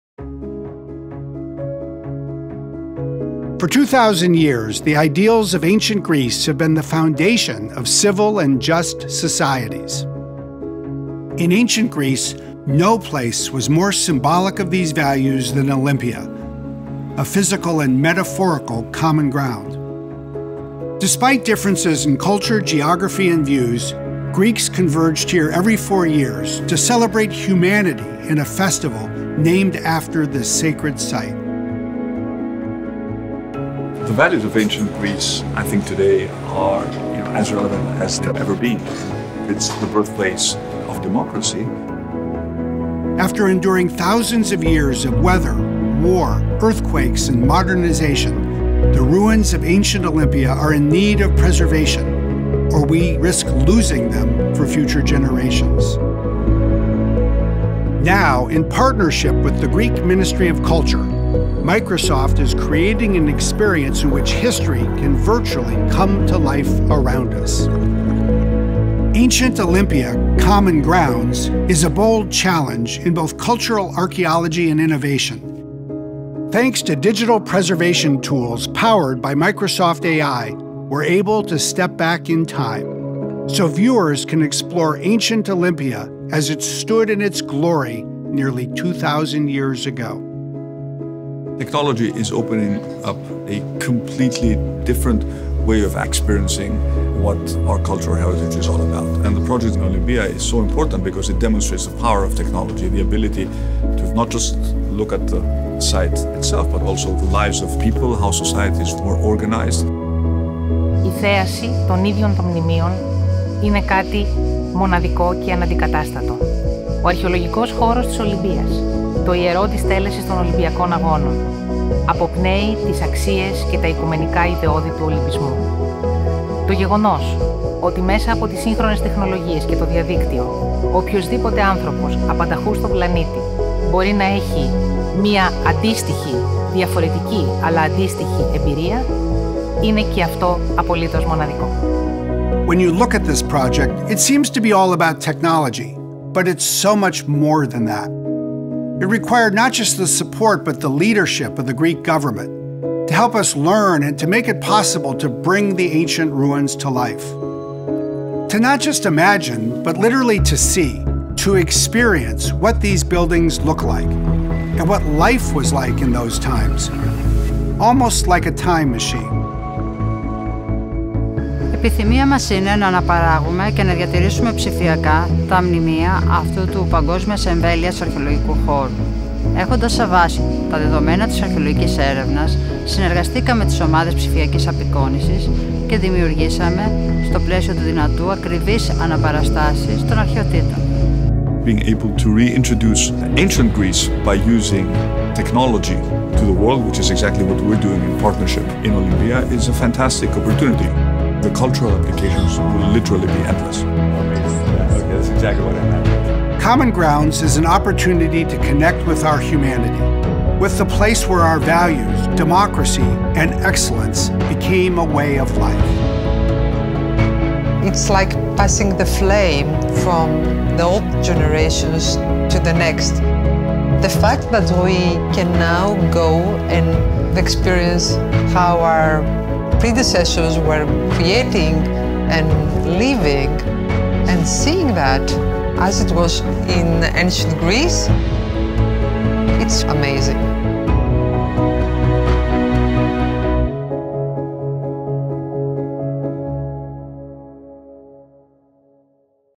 Experience Ancient Olympia as it stood 2,000 years ago with Common Grounds
Ancient Olympia: Common Grounds, a collaboration between Microsoft and the Hellenic Ministry of Culture and Sports, is a groundbreaking project that brings the site of Olympia in Ancient Greece to life as it stood over 2,000 years ago. The experience, built using Microsoft AI, digitally preserves the heritage of Ancient Olympia for generations to come.​